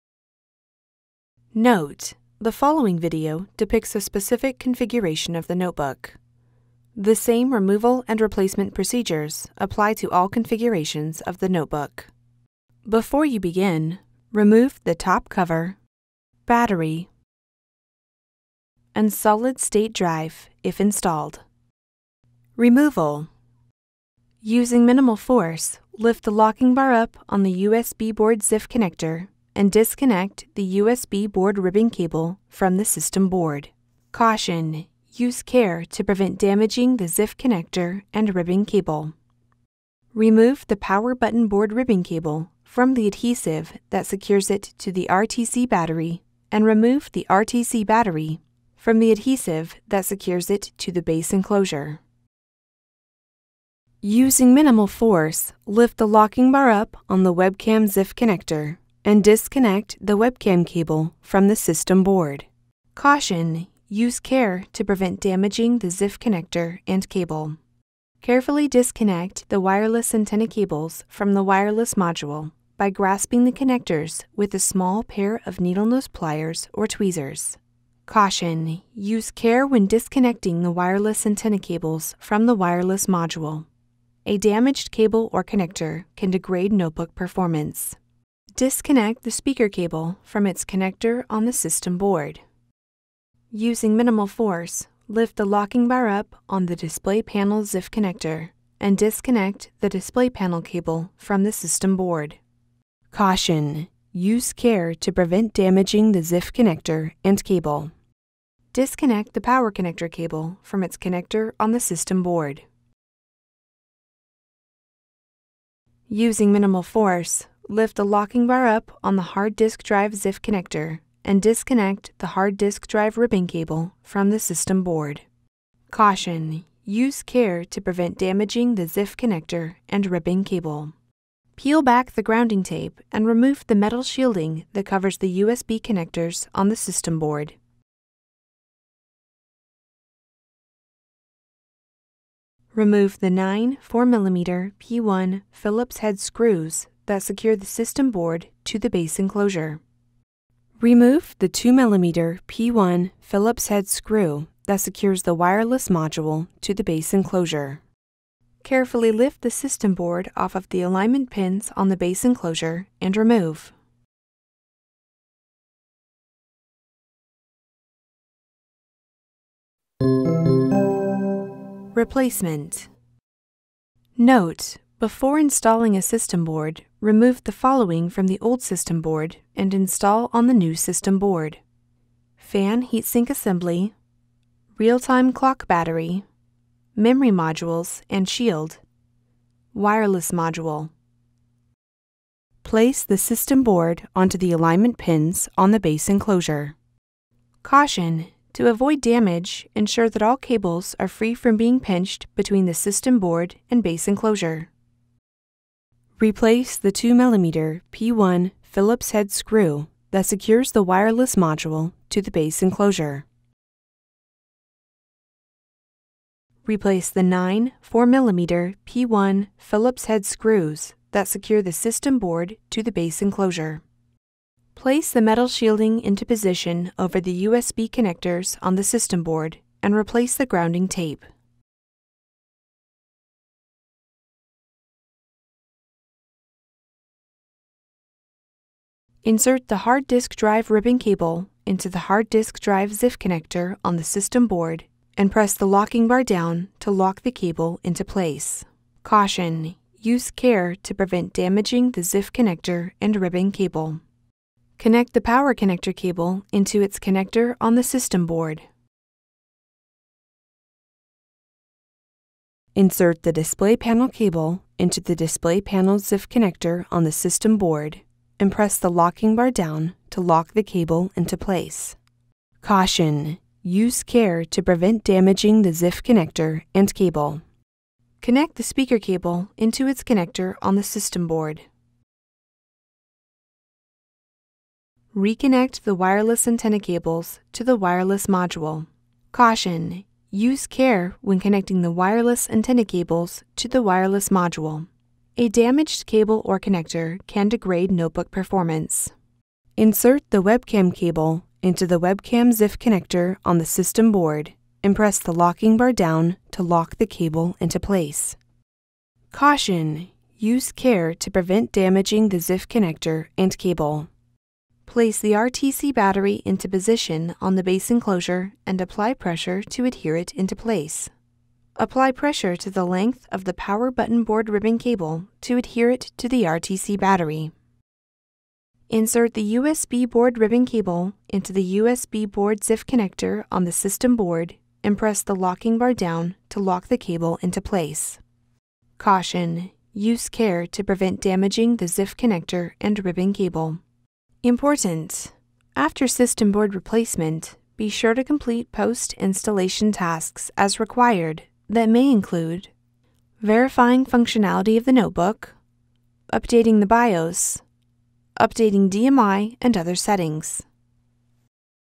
Replace the System Board | HP Pavilion x360 m3 Convertible Notebook | HP Support
Learn how to remove and replace the system board for HP Pavilion x360 m3 Convertible Notebook.

Chapters:
00:00 Introduction
00:21 Removal
02:49 Replacement